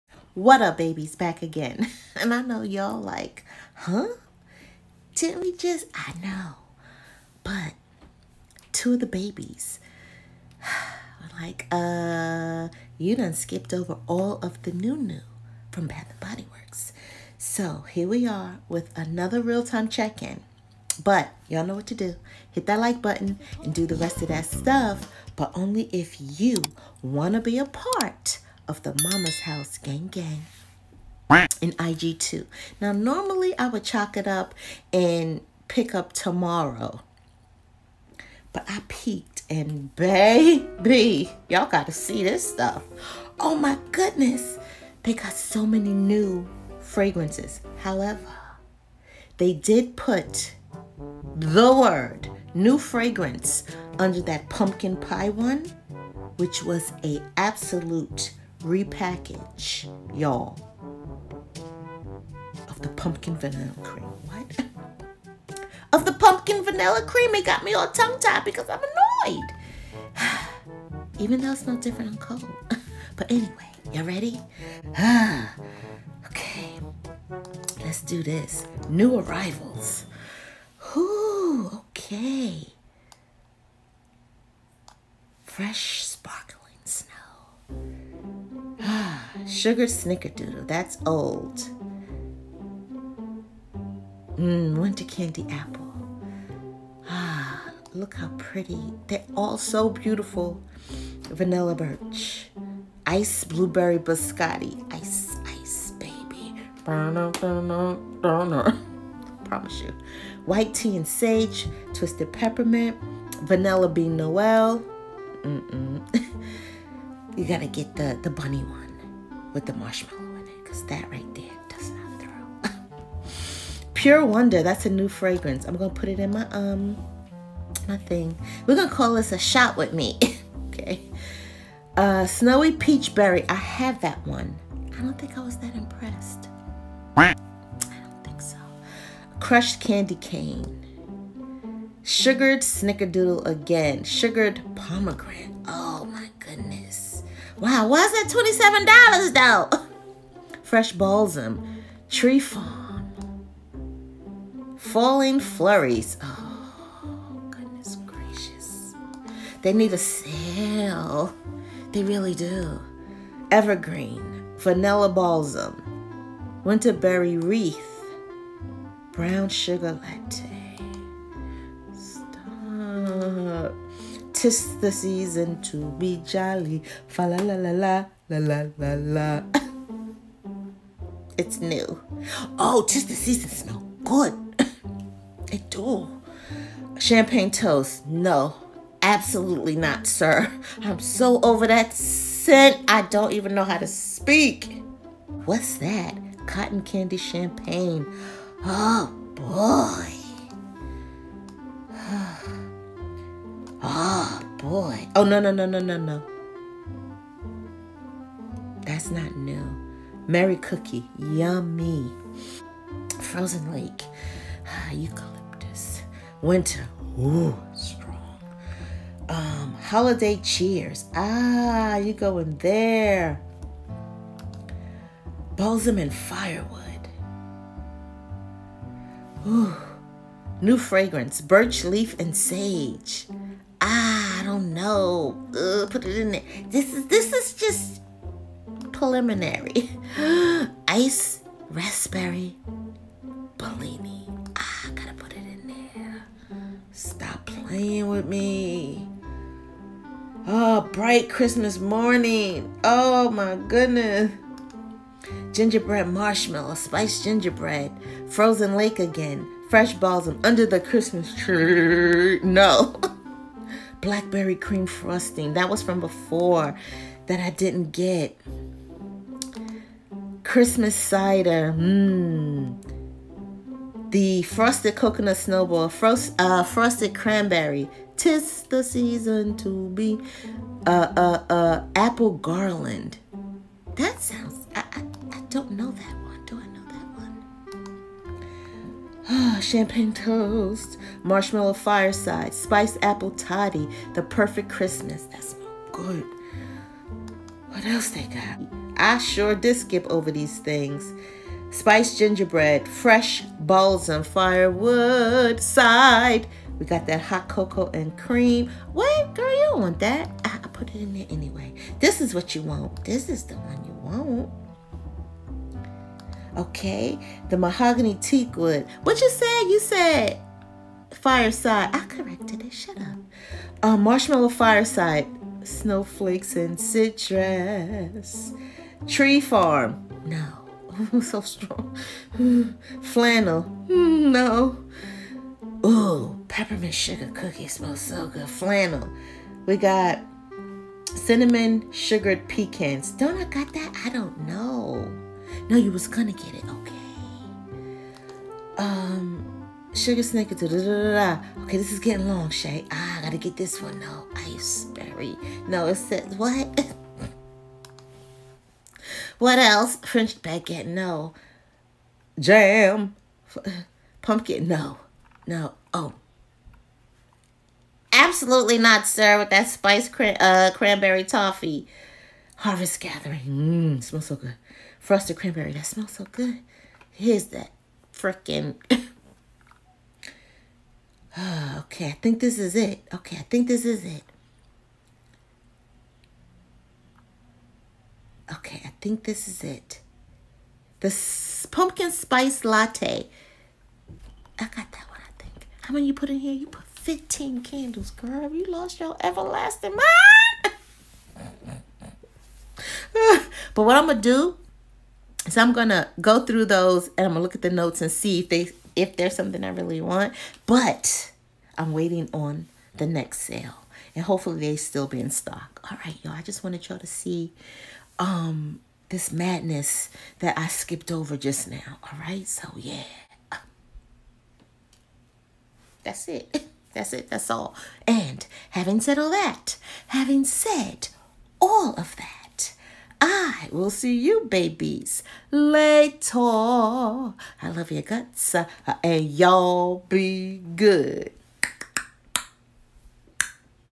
Bath and Body Works 🗣 THEY GOT NEW NEW HOLIDAY CANDLES... Come Shop With Mommas
Bath & Body Works:

LOTS OF NEW CHRISTMAS/HOLIDAY CANDLES

✔5/$20 or 8/$30 Hand Soaps
✔Buy 2, Get 2 Free All Aromatherapy Body Care
✔Regular Top Offers: